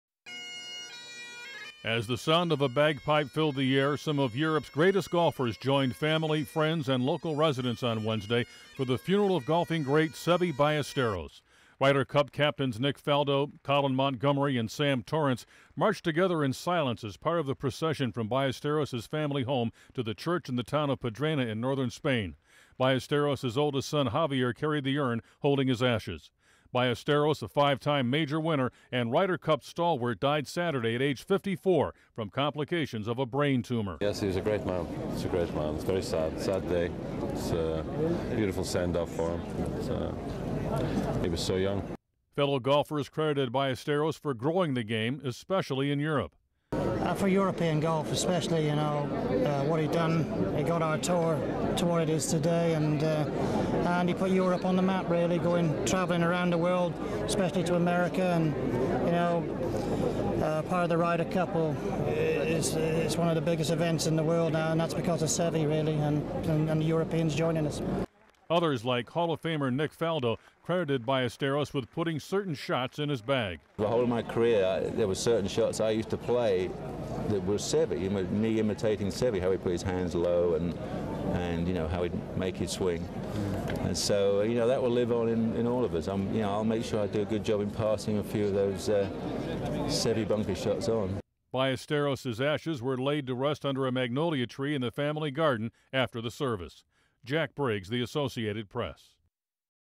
To the mournful wail of a lone bagpipe, some of Europe's greatest golfers joined family, friends and
(27 May 2011) HEADLINE: Golfers, family gather for Ballesteros funeral
CAPTION: To the mournful wail of a lone bagpipe, some of Europe's greatest golfers joined family, friends and local residents Wednesday at the funeral of Seve Ballesteros, paying an emotional final tribute to the Spaniard who revived the European game. (May 11)
Pedrena, Spain
APTN
AS BAGPIPES FILLED THE AIR, SOME OF EUROPES GREATEST GOLFERS JOINED FAMILY, FRIENDS AND LOCAL RESIDENTS ON WEDNESDAY FOR THE FUNERAL OF GOLFING GREAT SEVE BALLESTEROS.
RYDER CUP CAPTIANS NICK FALDO, COLIN MONTGOMERIE AND SAM TORRANCE MARCHED TOGETHER IN SILENCE AS PART OF THE PROCESSION FROM BALLESTEROS' FAMILY HOME TO THE CHURCH IN THE TOWN OF PEDRENA IN NORTHERN SPAIN. BALLESTEROS' OLDEST SON JAVIER CARRIED THE URN HOLDING HIS ASHES.
BALLESTEROS, A FIVE TIME MAJOR WINNER AND RYDER CUP STALWART DIED SATURDAY AT AGE 54 FROM COMPLICATIONS OF A BRAIN TUMOR.
Sam Torrance, golfer:
  "He was a great man, it is very sad, it is a sad day. It's a beautiful send-off for him. He was so young."
FELLOW GOLFERS CREDITED BALLESTEROS FOR GROWING THE GAME, ESPECIALLY IN EUROPE.
 Ian Woosnam, golfer:
  "For European golf especially, you know, what he done. He got the tour to what it is today and he put Europe on the map really, going travelling around the world, especially to America. Part of the Ryder Cup... it's one of the biggest events in the world now and that is because of Seve really and Europeans joining us."
OTHERS, LIKE HALL OF FAMER NICK FALDO, CREDITED BALLESTEROS WITH PUTTING CERTAIN SHOTS IN HIS BAG.
Nick Faldo, golfer:
  "For the whole of my career there were certain shots that I used to play that was Seve, it was me imitating Seve. How he would put his hands low, how he would make his swing. That will live on in all of us. I'll make sure I'll do a good job in passing a few of those Seve bunker shots on."
BALLESTEROS' ASHES WERE LAID TO REST UNDER A MAGNOLIA TREE IN THE FAMILY GARDEN AFTER THE SERVICE.